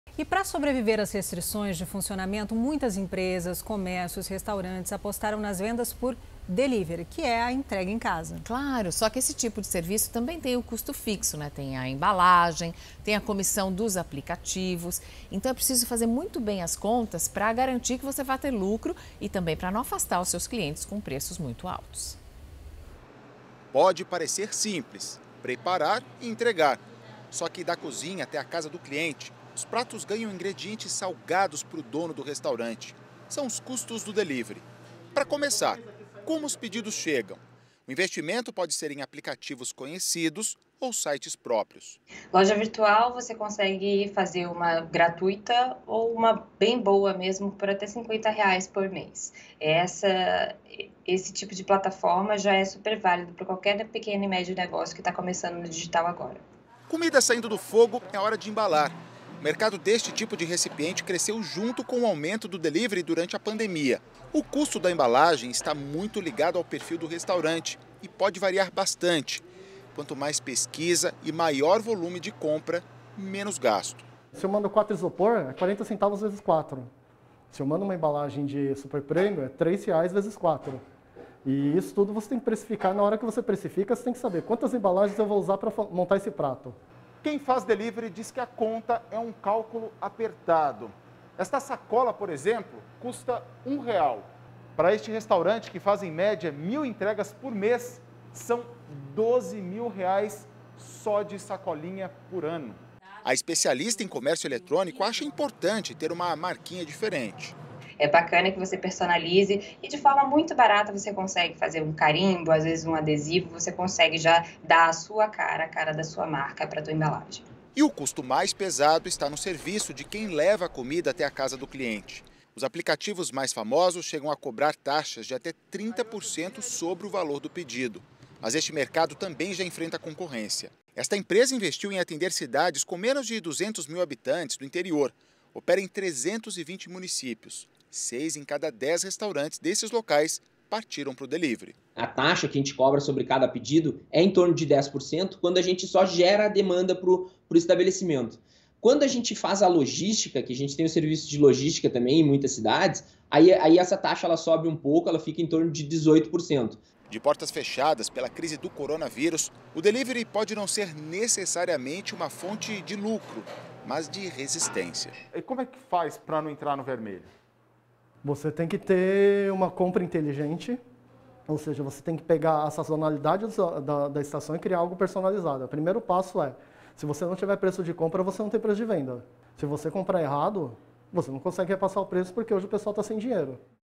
Custos de embalagens podem encarecer entregas por delivery
Para sobreviver às restrições de funcionamento, muitas empresas, comércios e restaurantes passaram a apostar nas vendas por delivery. Mas esse tipo de serviço tem um custo fixo, com embalagens e comissão dos aplicativos, por isso é preciso estimar os gastos para garantir o lucro e não afastar os clientes com preços mais altos.